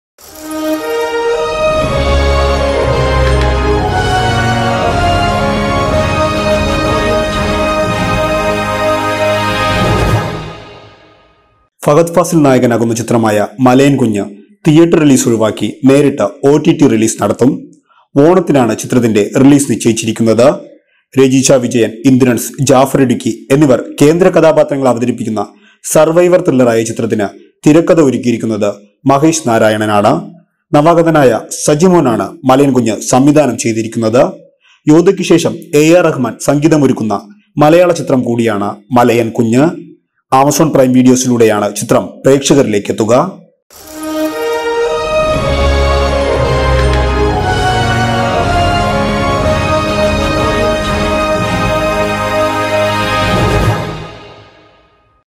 BLive Filmy Funda | തിയേറ്റർ കാണാതെ മലയൻകുഞ്ഞ്
ഫഹദ് ഫാസിൽ നായകനാകുന്ന ചിത്രമായ മലയൻകുഞ്ഞ് തിയേറ്റർ റിലീസ് ഒഴിവാക്കി നേരിട്ട് ott റിലീസ് നടത്തും. ഓണത്തിനാണ് ചിത്രത്തിന്റെ റിലീസ് നിശ്ചയിച്ചിരിക്കുന്നത്. രജീഷ വിജയൻ, ഇന്ദ്രൻസ്, ജാഫർ ഇടുക്കി എന്നിവർ കേന്ദ്രകഥാപാത്രങ്ങളെ അവതരിപ്പിക്കുന്ന സർവൈവൽ ത്രില്ലർ ആയ ചിത്രത്തിന് തിരക്കഥ ഒരുക്കിയിരിക്കുന്നത്  മഹേഷ്‌ നാരായണനാണ്. നവാഗതനായ സജിമോനാണ് മലയൻകുഞ്ഞ് സംവിധാനം ചെയ്തിരിക്കുന്നത്. യോദ്ധയ്ക്ക് ശേഷം എ. ആർ റഹ്മാൻ സംഗീതമൊരുക്കുന്ന മലയാളചിത്രം കൂടിയാണ് മലയൻകുഞ്ഞ്. ആമസോൺ പ്രൈം വീഡിയോസിലൂടെയാണ് ചിത്രം പ്രേക്ഷകരിലേയ്ക്കെത്തുക.